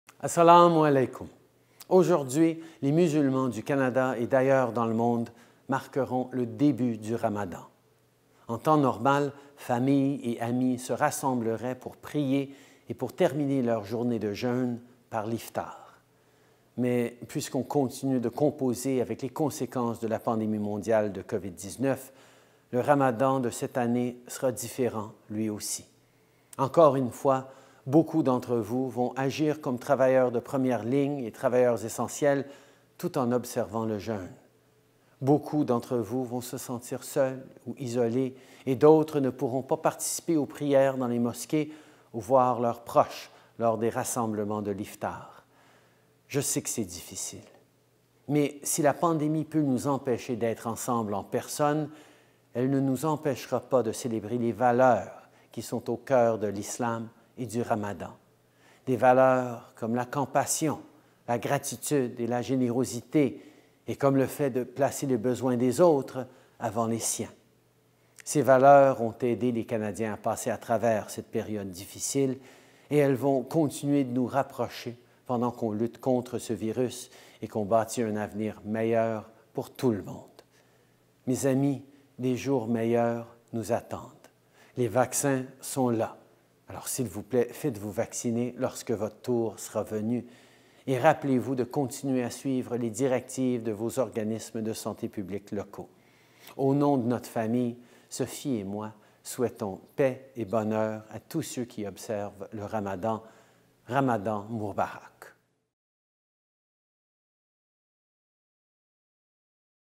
Le premier ministre Trudeau offre ses vœux à l’occasion du ramadan
12 avril 2021
Le premier ministre Justin Trudeau souhaite paix et bonheur à tous ceux et celles qui célèbrent le ramadan.